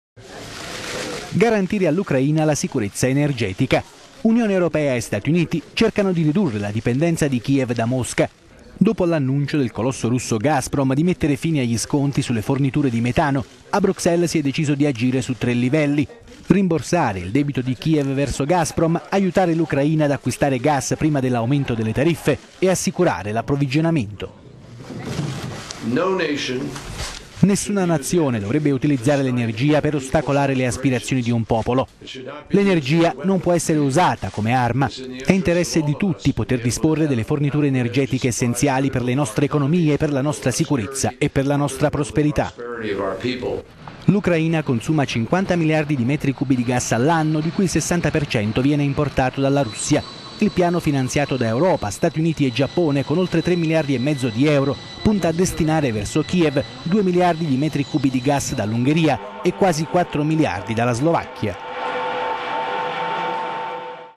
Ucraina: vertice Usa e Ue per superare dipendenza energetica da Mosca
Garantire all'Ucraina la sicurezza energetica. Il consiglio energetico di Unione europea e Stati Uniti cerca di ridurre la dipendenza di Kiev da Mosca.

Dopo "l'annuncio del colosso russo Gazprom" di mettere fine agli sconti sulle forniture di metano, a Bruxelles si è deciso di agire su tre livelli: rimborsare il debito di Kiev verso Gazprom, aiutare l'Ucraina ad acquistare gas prima dell'aumento delle...